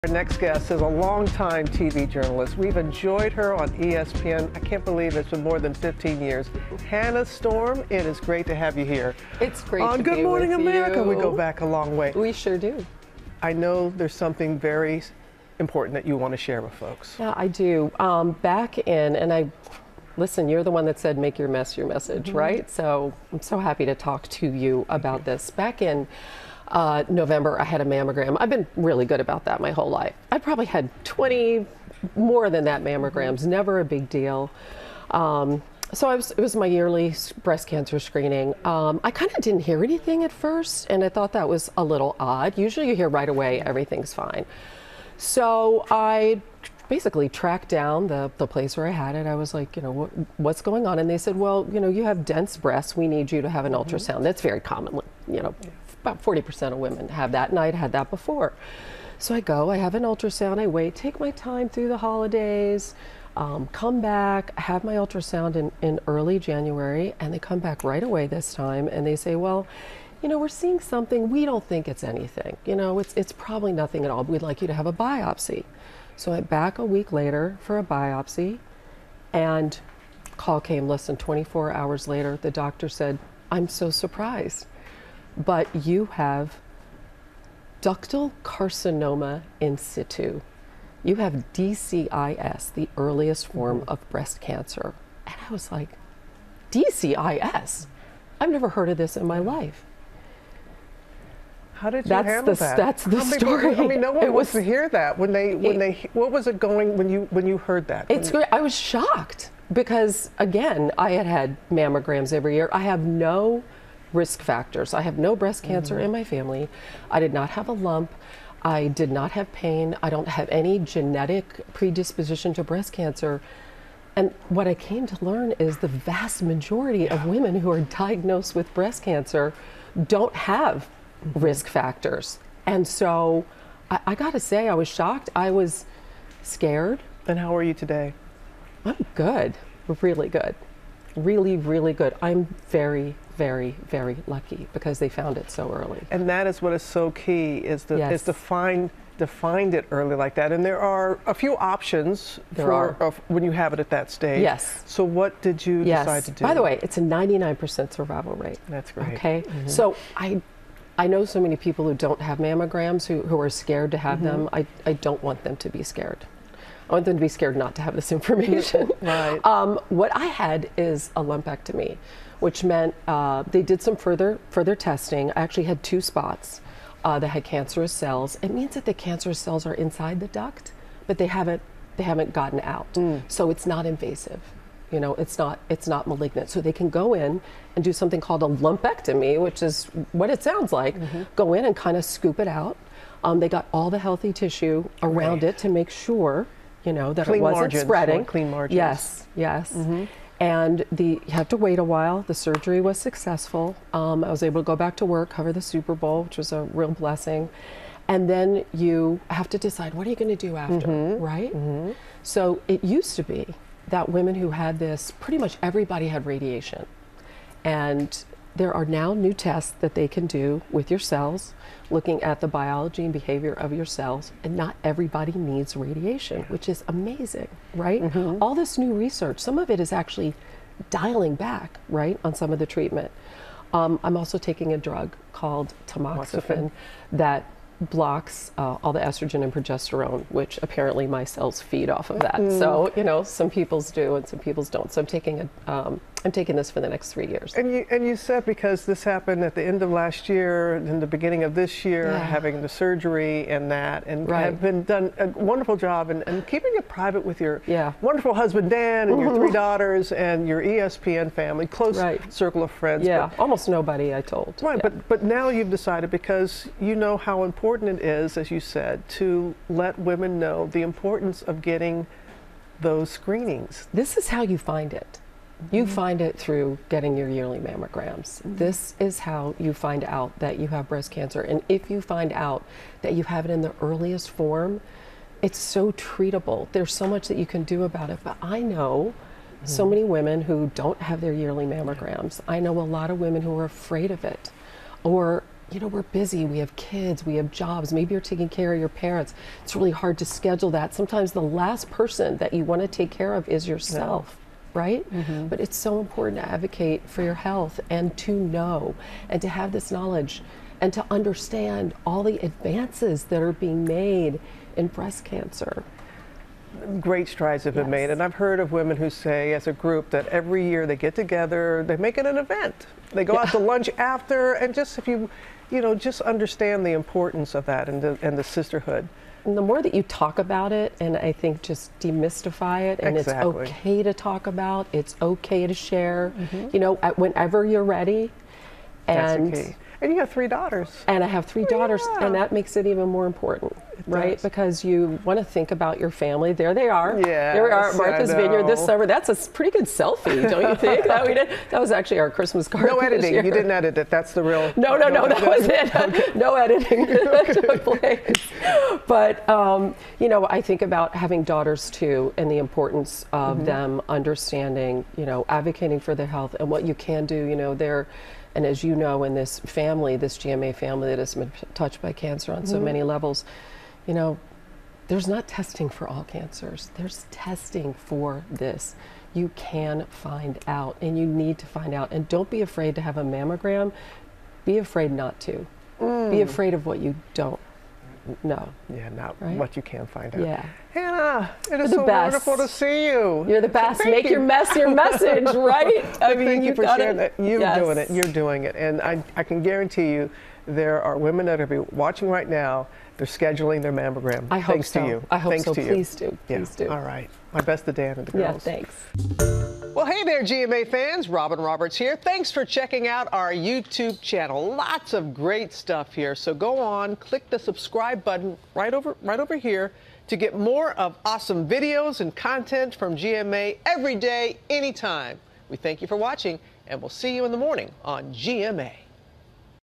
Hannah Storm opens up about breast cancer diagnosis
The co-anchor of "SportsCenter," who is now cancer-free, revealed publicly for the first time that she was diagnosed with breast cancer in January.